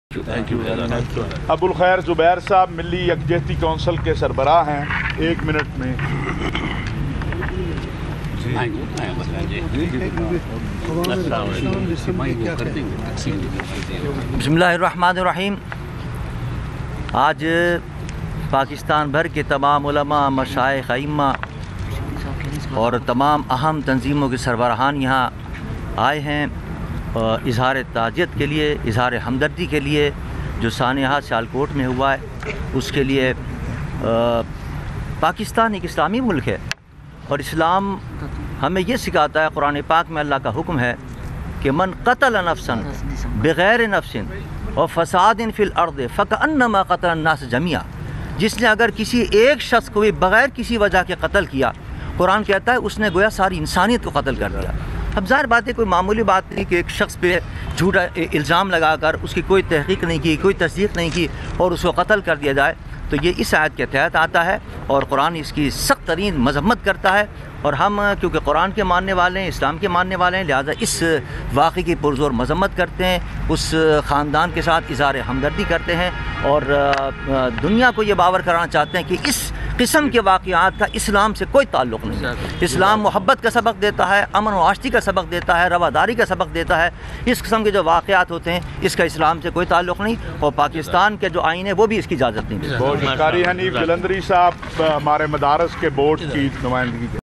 Dr Sahibzada Zubair Alvari Point of View on Sialkot Incident
Priyantha Kumara Diyawadana was a 49 year old, Sri Lankan citizen lynched by a mob on December 3, 2021 in Sialkot, Pakistan over allegations of blasphemy. ... He had been accused of blasphemy for removing a poster featuring religious content.But JUP N and Mili Yakjahti Council President Dr Sahibzada Abul Khair Muhammad Zubair Alwari Condemn This Issue